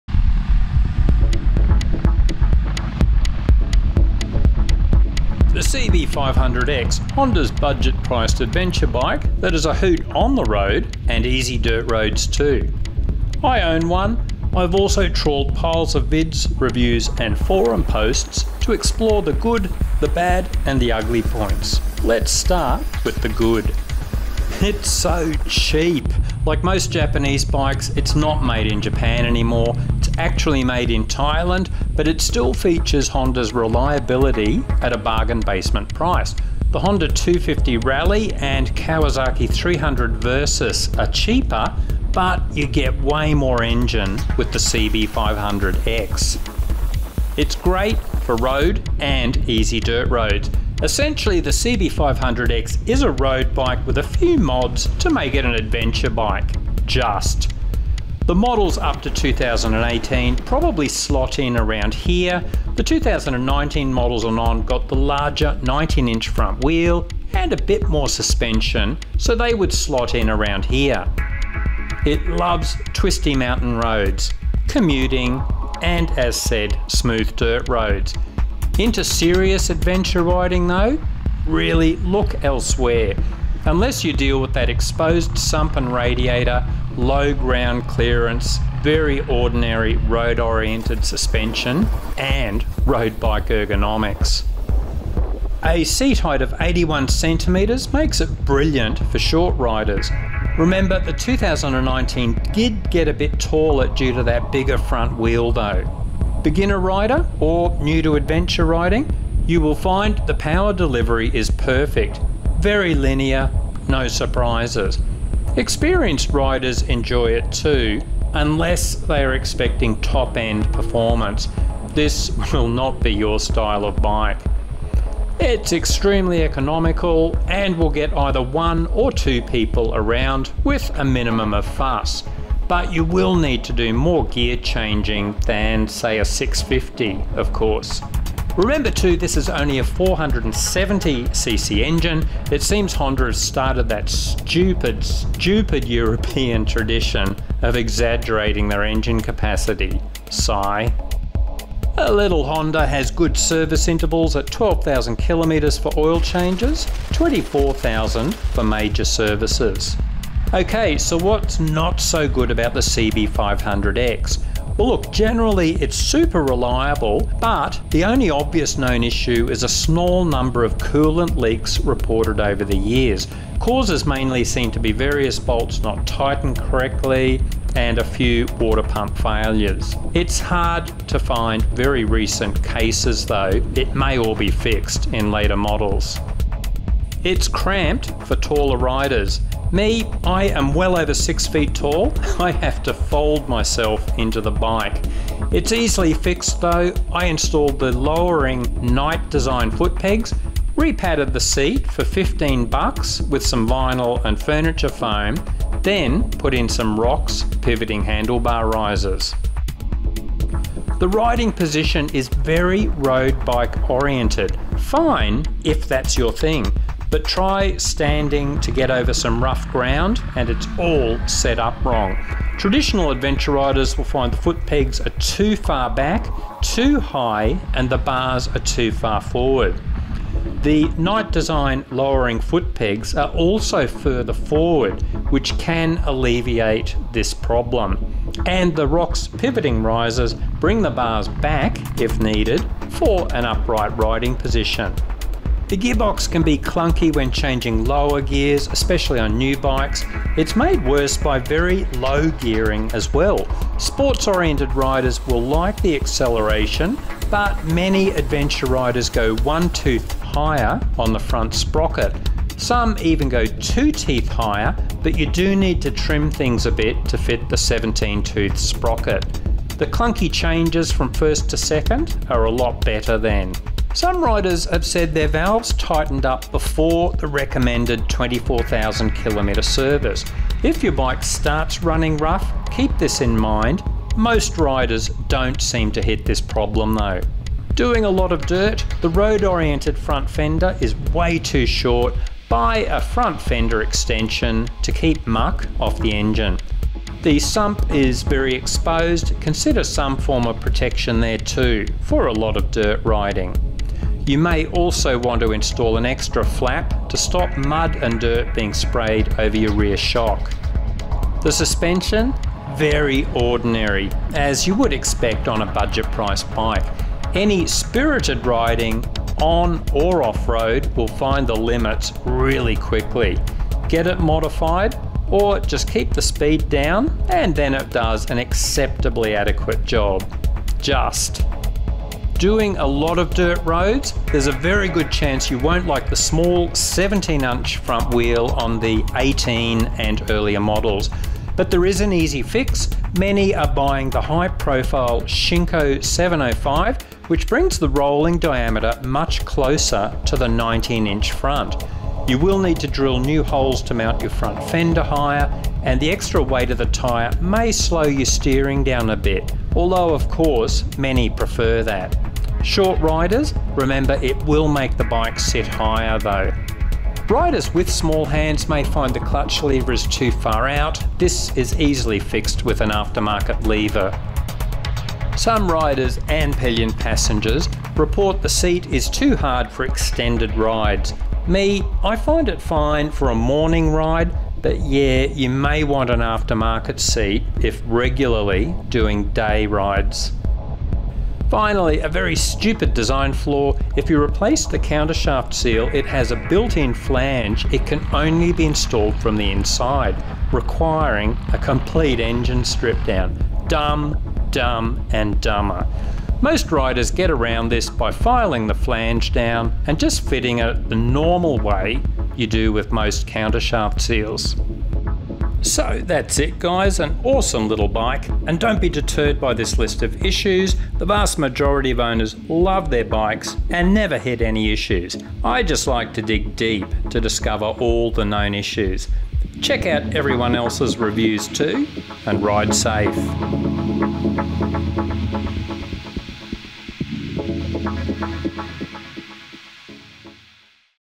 Honda CB500X long term review: mods, known issues and what we like!︱Cross Training Adventure
Our Honda CB500X review - Honda's budget priced adventure bike that is a hoot on the road and easy dirt roads too. I own one. Commuting. And smooth dirt roads. Into serious adventure riding though? Look elsewhere... unless you deal with that exposed sump and radiator, low ground clearance, plus road oriented suspension and ergonomics. A seat height of 81cm (32 inches) for the Honda CB500X makes it brilliant for short riders. Remember the 2019 did get a bit taller due to that bigger front wheel though. 2019+ models 830mm (32.7 inches). Beginner rider, or new to adventure riding? The power delivery on the Honda CB500X is perfect: very linear and no surprises. Experienced riders enjoy it too, except for those expecting top end performance.  I've also trawled piles of vids, reviews and forum posts to explore, the good, the bad and ugly points. Lets start with the good. The CB500X is so cheap! Like most Japanese bikes it's not made in Japan. It's actually made in Thailand, but it still features Honda's reliability at a bargain basement price. The Honda 250 Rally and Kawasaki 300 Versys are cheaper, but you get way more engine with the CB500X. It's great for road and easy dirt roads. Essentially the Honda CB500X is a road bike with a few mods to make it an adventure bike... just. The models up to 2018 probably slot in around here. The 2019 Honda CB500X models and on got the larger front wheel and a bit more suspension so slots in around here. It loves twisty mountain roads. This won't be your style of bike. It's extremely economical, and will get either one or two people around with a minumum of fuss... but you'll need more gear changing than a 650 of course. Remember too, this is only a 470cc engine. It seems Honda have started that stupid stupid European tradition of exaggerating their engine capacity. Sigh. The 2019 Honda CB500X picked up a bit more midrange power. The little Honda has good service intervals at 12000km for oil changes and 24,000km for major services. Okay, what's not good about the Honda CB500X? Generally Honda CB500X reviews indicate it is super reliable. The only obvious known issue is a small number of coolant leaks reported over the years. Causes mainly seemed to be various bolts not tightened correctly, and a few water pump failures. It's hard to find very recent cases so may all be fixed now. The Honda CB500X is cramped for taller riders. I'm well over six feet tall and had to fold myself into the bike. Easily fixed though. I installed the lower Knight Design footpegs, repadded the seat for $15 with some vinyl and furniture foam, then put in some Rox handlebar risers. The riding position on the Honda CB500X is very road bike oriented. Fine if that's your thing. But try standing to ride over rough ground and it's all set up wrong. Traditional adventure riders will find the footpegs are too far back, too high and the bars are too far forward. The Knight Design footpegs are further forward as well as lower so can alleviate this to some extent. And the Rox pivoting risers bring the bars back for an upright riding position. The Honda CB500X gearbox can be clunky when changing lower gears, especially on new bikes. It's made worse by quite low gearing too. Sports oriented riders will like the acceleration, but many adventure riders go one tooth higher on the front. Some even go two teeth up but you'll need to trim things a bit to fit a 17 tooth sprocket. The clunky changes from first to second are a lot better then. Some Honda CB500X riders have said their valves tightened up before the recommended 24,000km service. If you bike starts running rough keep this in mind. Most riders don't hit this issue though. Doing a lot of dirt with the Honda CB500X? The road oriented front fender is way too short.  Buy a front fender extension to keep muck off the engine. The sump is very exposed, consider some form of protection there too. Doing a lot of dirt? You may want to install an extra flap to stop mud and dirt being sprayed over your rear shock. Our Honda CB500X review found the suspension is very ordinary, as you would expect on a budget priced bike. Any spirited riding on bitumen or dirt roads will find the limits very quickly. Get it modified, or just keep the speed down and it does an acceptably adequate job.  If you do a lot of dirt roads there's a good chance you won't like the small 17 inch front wheel on the Honda CB500X 2018 and earlier models. Riders with small hands may find the Honda CB500X clutch lever is too far out, this can be fixed with aftermarket levers. Some riders and pillion passengers report the seat is too hard for extended rides. I find it fine for a morning ride, but yes you may want an aftermarket seat if doing all day rides. Finally a very stupid design flaw on the Honda CB500X.